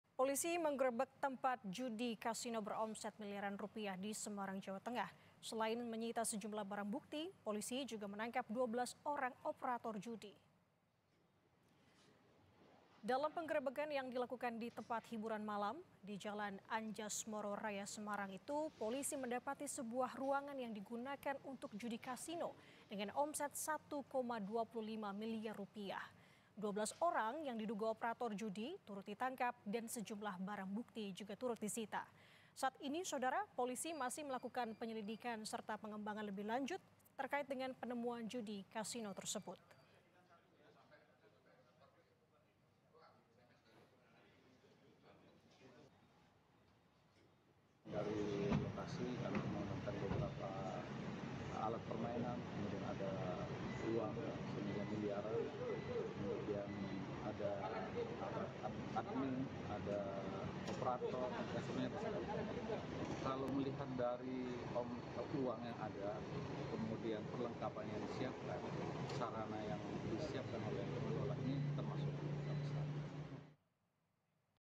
Polisi Gerbek Tempat Judi Kasino di Semarang Omzet Rp1,25 Miliar
KOMPAS.TV - Polisi Gerbek Tempat Judi Kasino di Semarang Omzet Rp1,25 Miliar.